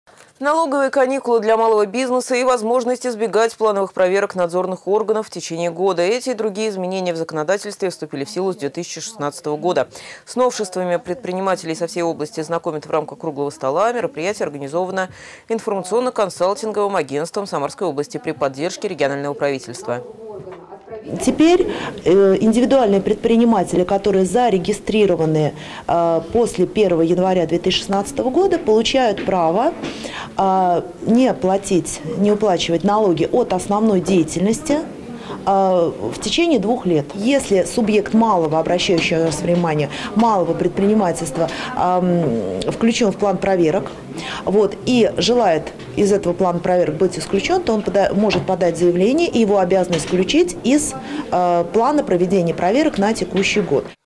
Изменения в налоговом законодательстве обсудили на круглом столе для предпринимателей
Налоговые каникулы для малого бизнеса и возможность избегать плановые проверки надзорных органов в течение года. Эти и другие изменения в законодательстве вступили в силу с 2016 года. С новшествами предпринимателей со всей области знакомят в рамках круглого стола. Мероприятие организовано информационно-консалтинговым агентством Самарской области при поддержке регионального правительства.